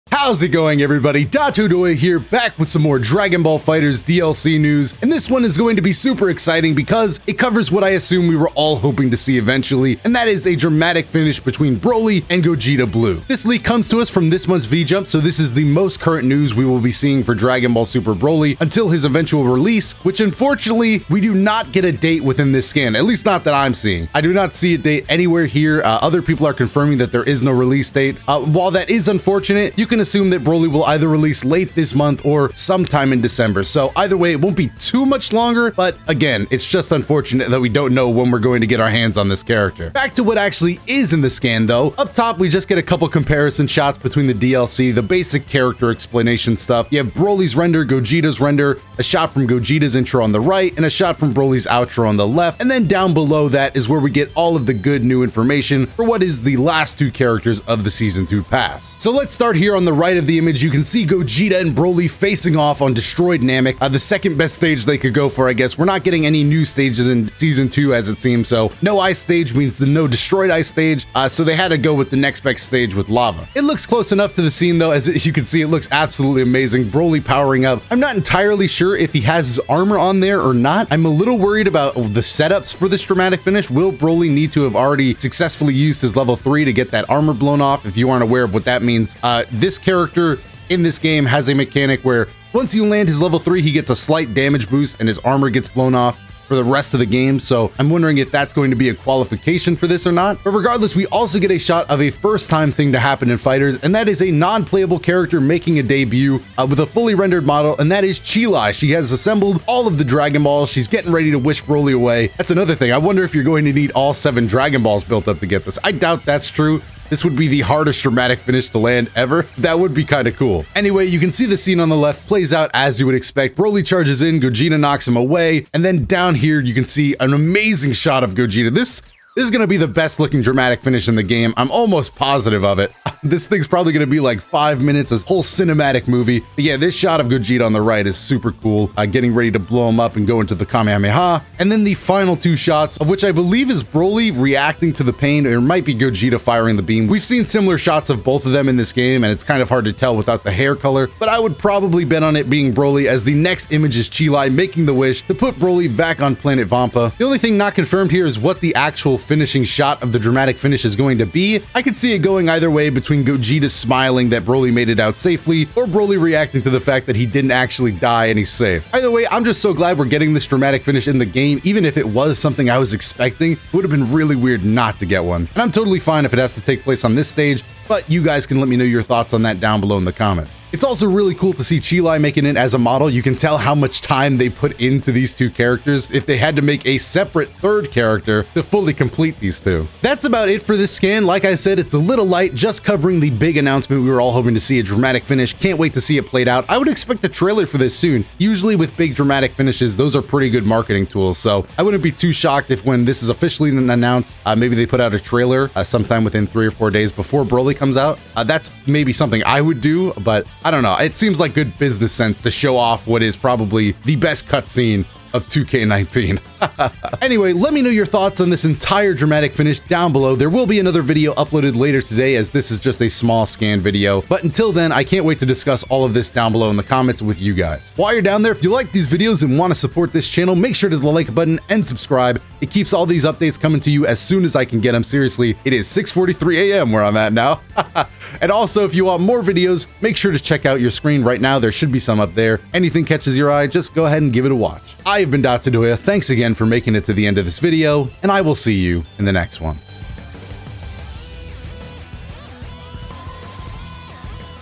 NEW DBS BROLY DRAMATIC FINISH REVEALED! | Dragonball FighterZ DLC News
The brand New Dramatic Finish for dragonball fighterz has just been revealed and it features DBS Broly, Gogeta Blue, and Cheelai! This video attempts to break down the dbfz leaks and showcase the scans for the dramatic finish!

Dragonball fighterz dlc reaction and analysis!